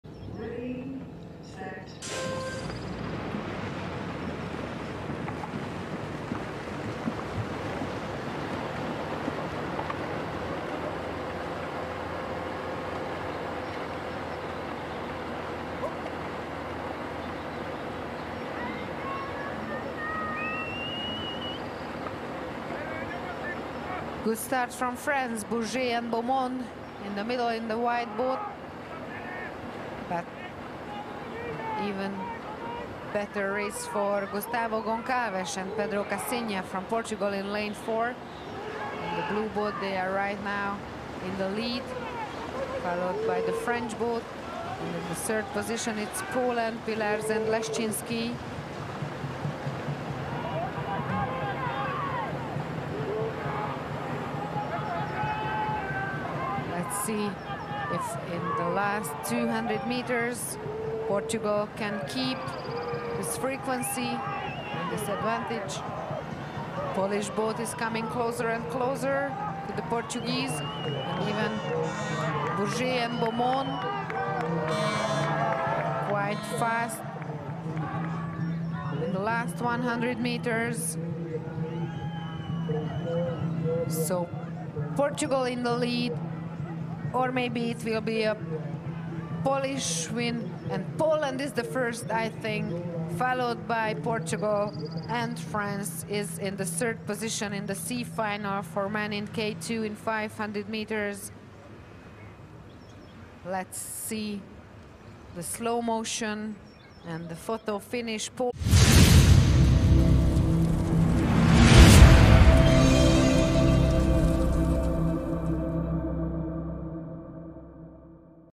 K2 Men 500m - Final C / 2024 ICF Canoe-Kayak Sprint World Cup
Canoe-Kayak Sprint European Paris Olympic Qualifier 🇫🇷
2024 ICF Canoe-Kayak Sprint World Cup 🌎🏆
2024 ICF Canoe-Kayak Paracanoe World Championships & Paris Paralympic Qualifier 👑
8–12 May Szeged Hungary 🗓️🇭🇺